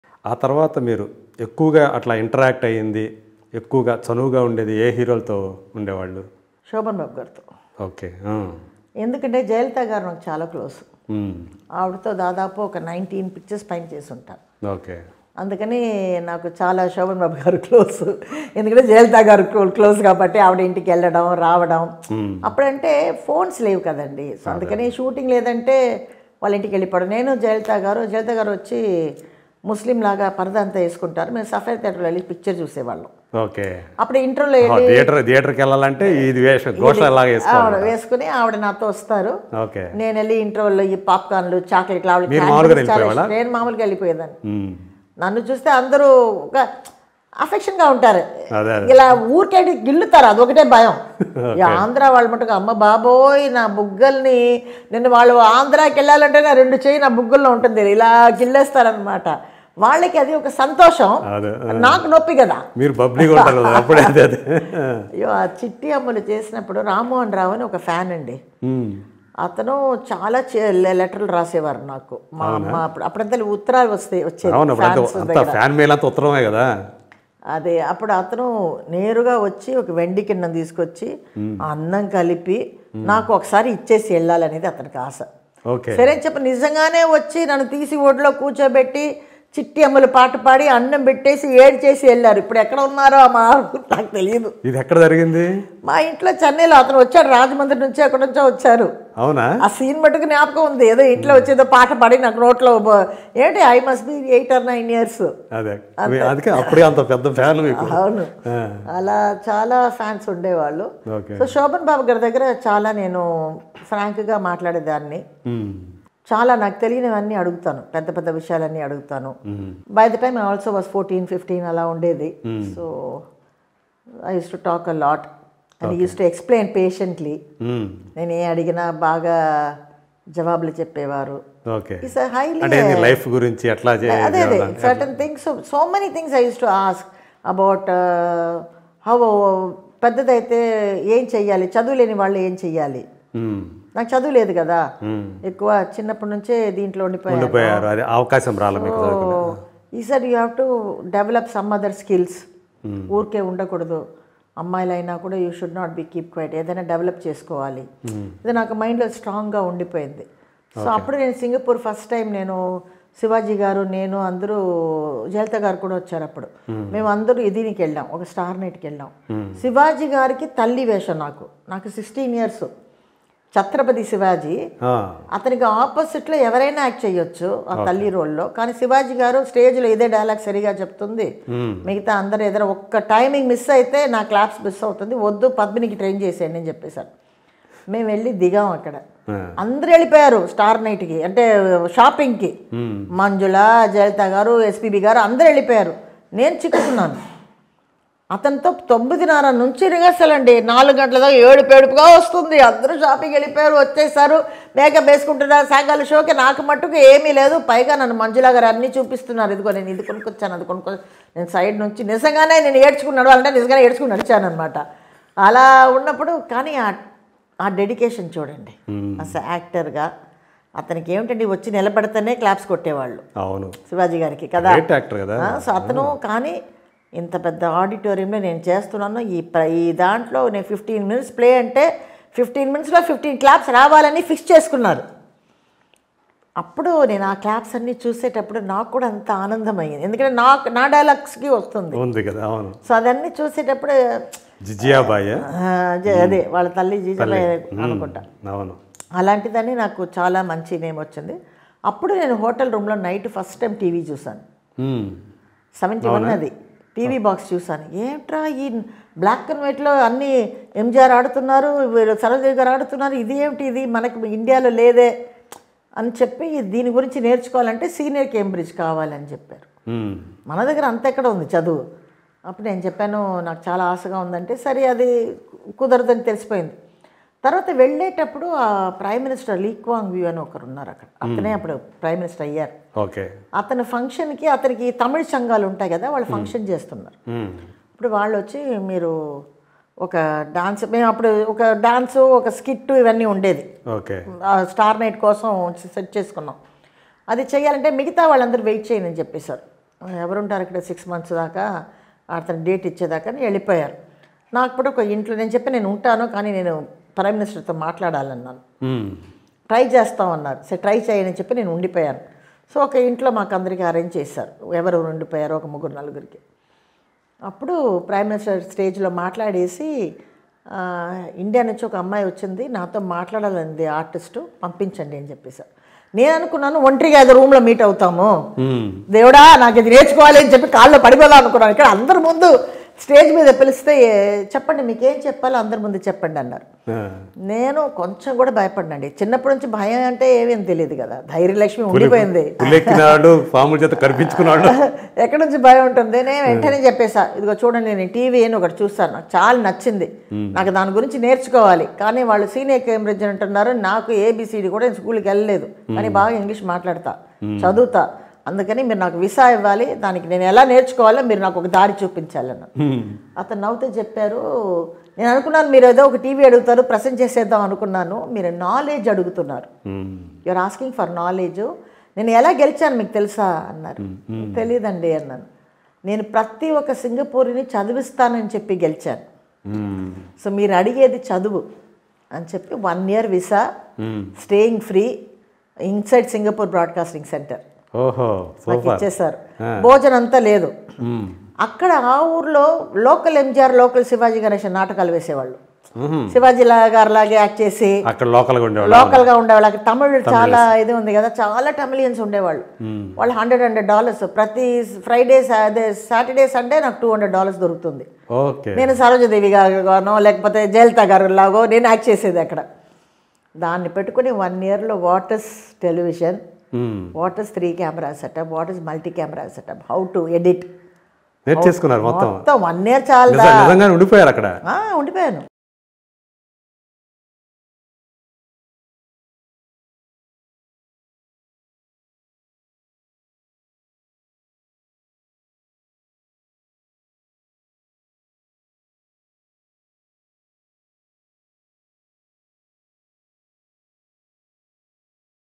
Actress Kutty Padmini Interview | నాకు జయలలిత బాగా క్లోజ్.. వాళ్ళ ఇంటికెళ్తుండడం వల్ల.. TeluguOne
Senior Actress Kutty Padmini Exclusive Interview by Senior Journalist Yagna Murthy Buddhi. Indian Actress Kutty Padmini Latest Interview.